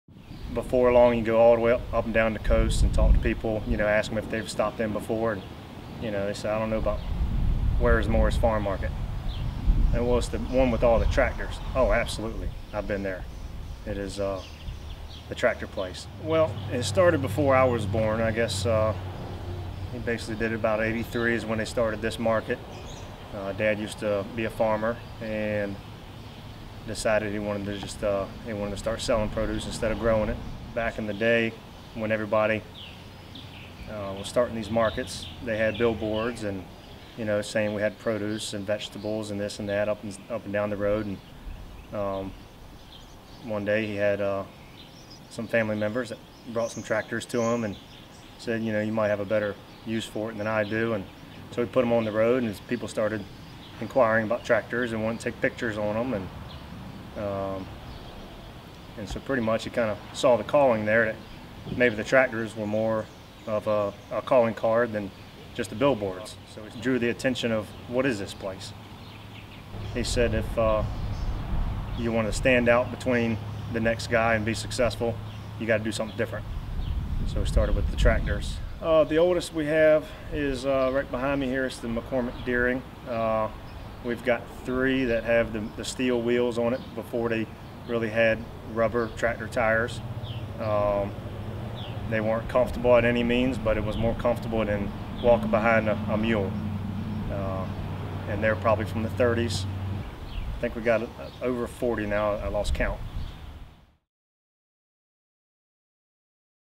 Morris Farm Market
The Morris family started the tractor collection in 1983, and now have over 40 tractors at Morris Farm Market. The market is known for their collection of tractors, so they use them as advertisement along Route 168.